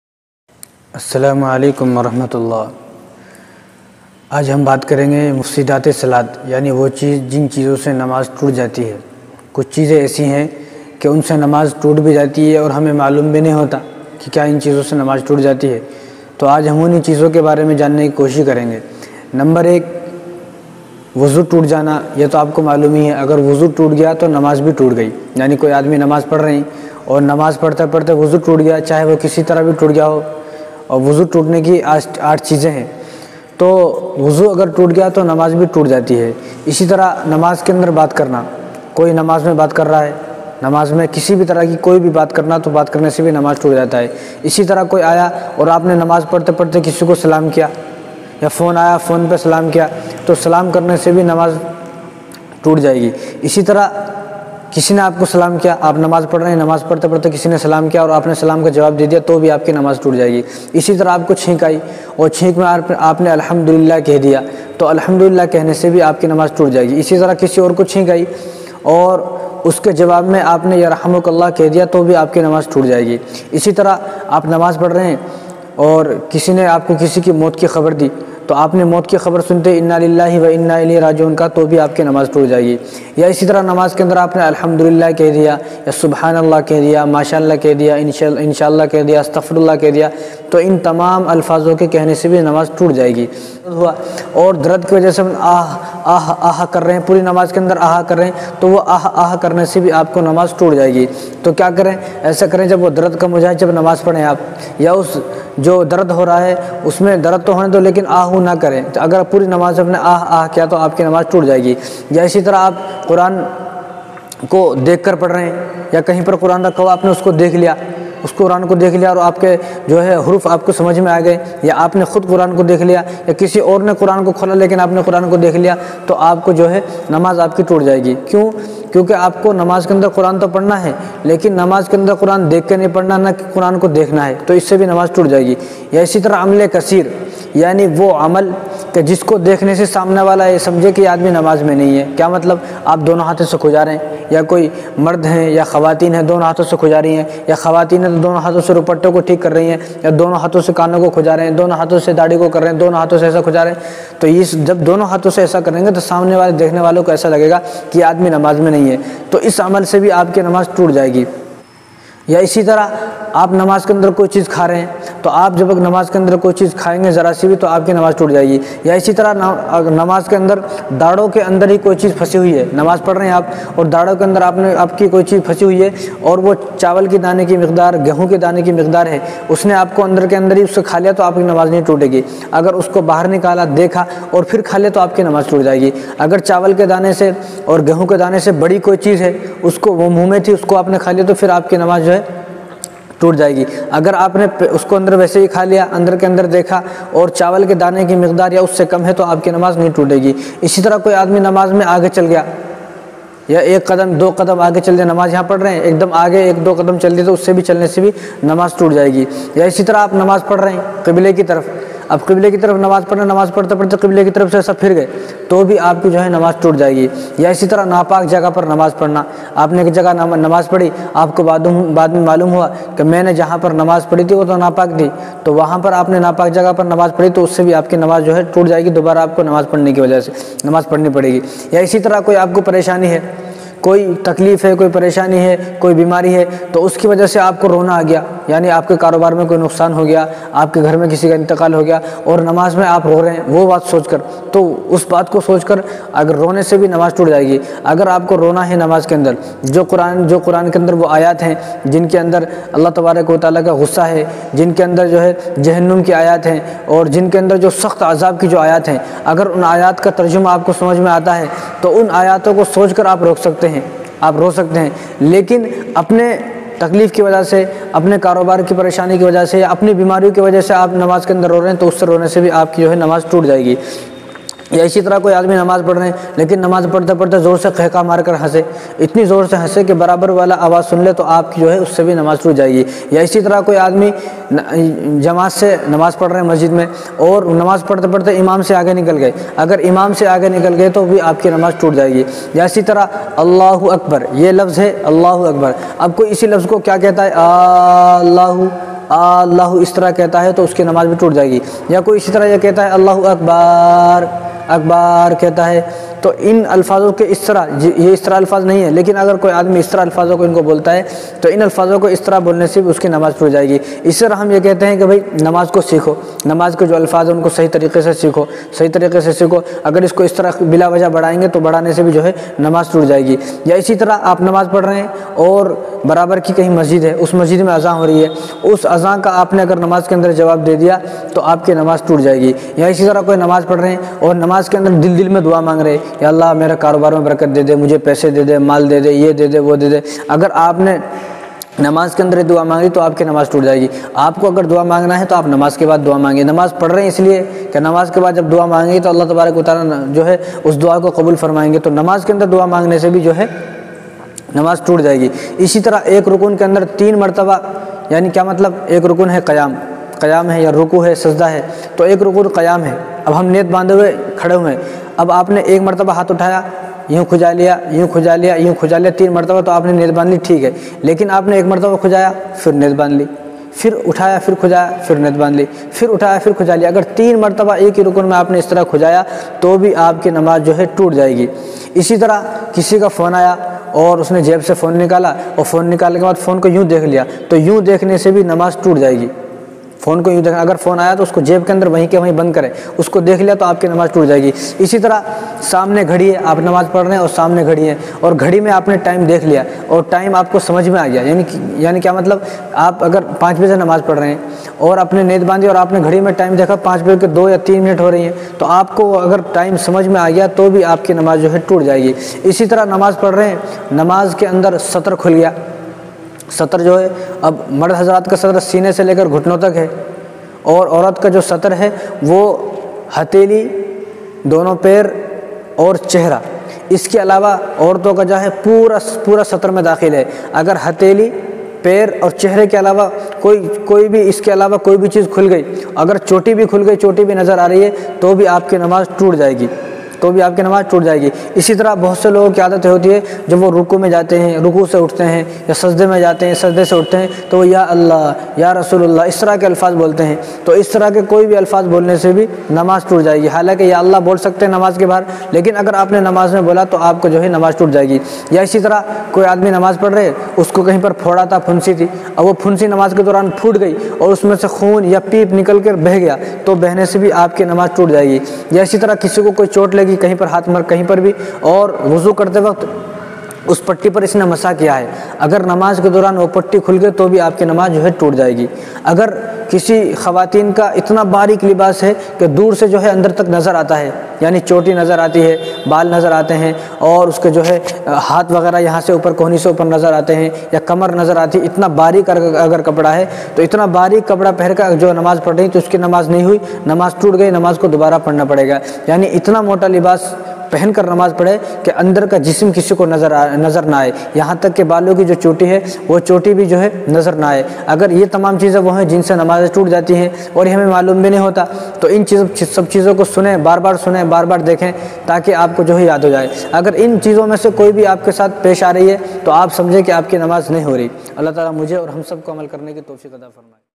namaz kin chezon se tot jati h namaz ko tod ne wali chezen / by md inaam / md inaam bayans
mufsidat e salat md inaam bayans